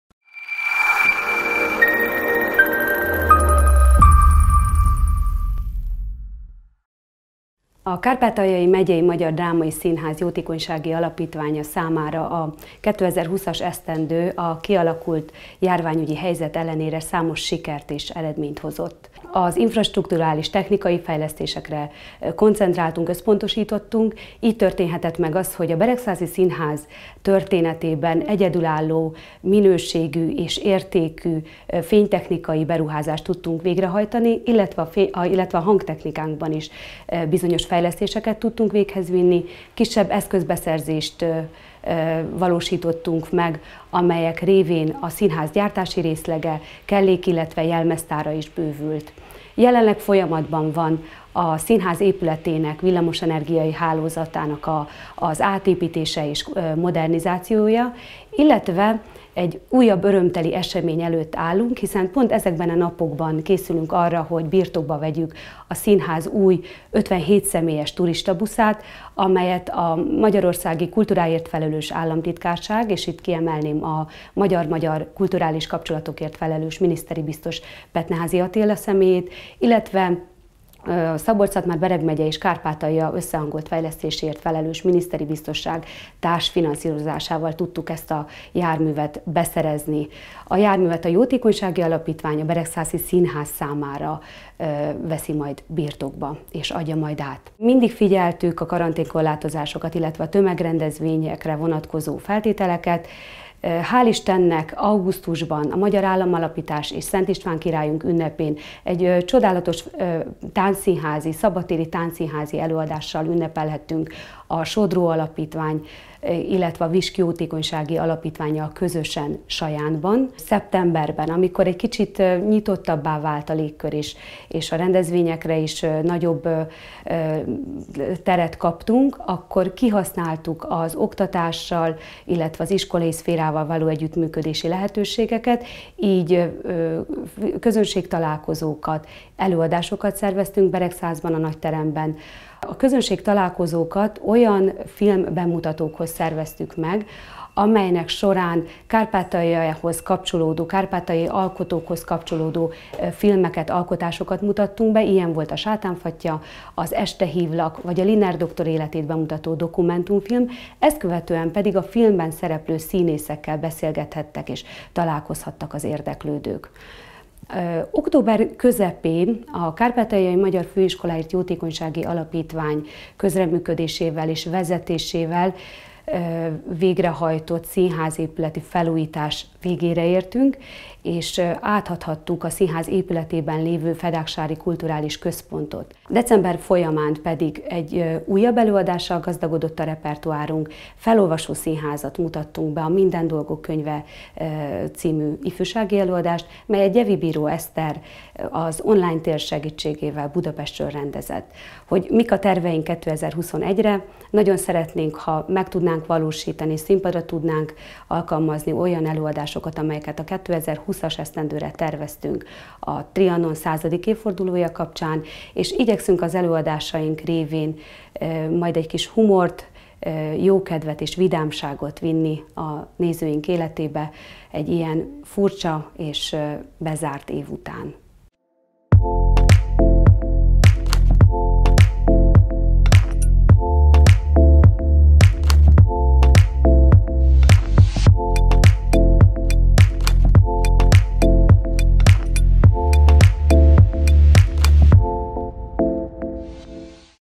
TV21 UNGVÁR - Kommentár nélkül (2020.12.30.)02
Minden nehézség ellenére fejlődik a beregszászi színház